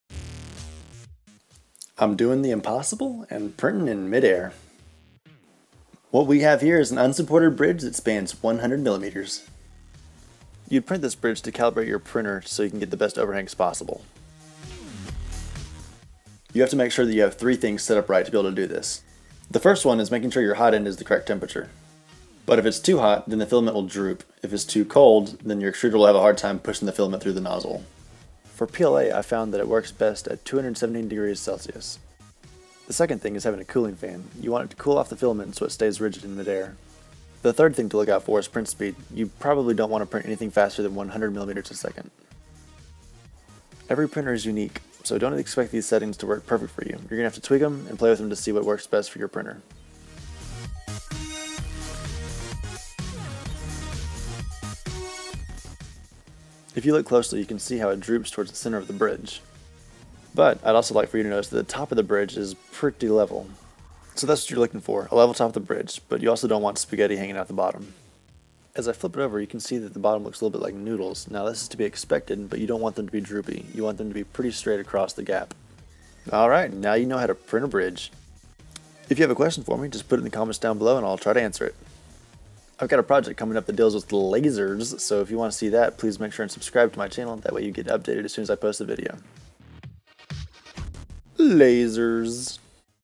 ULTIMAKER Print # 9 THE 100mm Bridge
I have  been asked to make a video explaining how it is possible to print over a gap without supports. This video goes into the specifics on what printer settings you need to tweak and how the printer is capable of spanning gaps.

Material: PLA
Temperature:
Hot End - 217 C
Heated Bed - NA
Layer Height: 0.1 mm
Infill: 40%
Speed: 100 mm/s